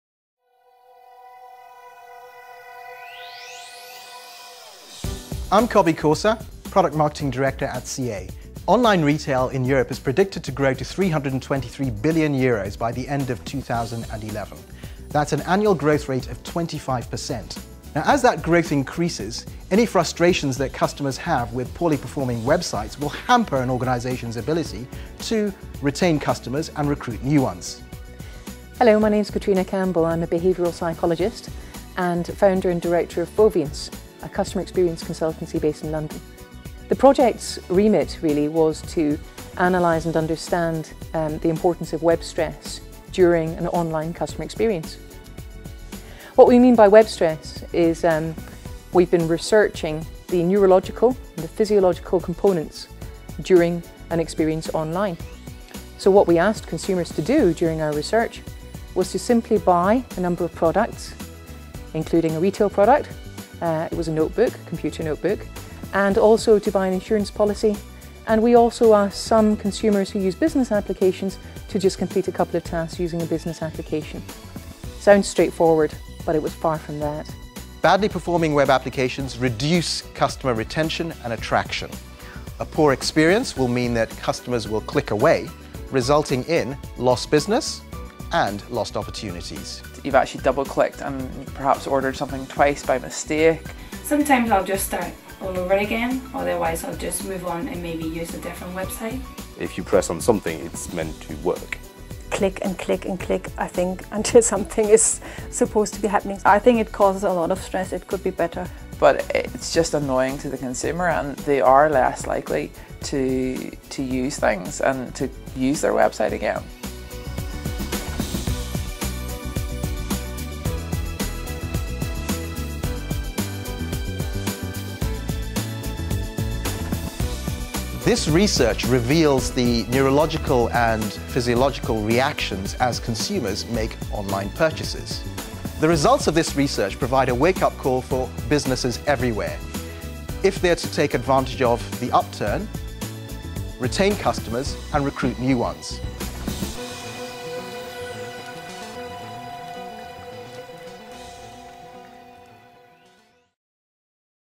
Web Stress is bad for business
CA partnered with Foviance - a leading customer experience consultancy - to explore web stress in relation to application performance, and its impact on consumer behaviour and buying habits.  Brain wave analysis from the experiment revealed that participants had to concentrate up to 50% more when using badly performing websites, while facial muscle and behavioural analysis of the subjects also revealed greater agitation and stress in these periods.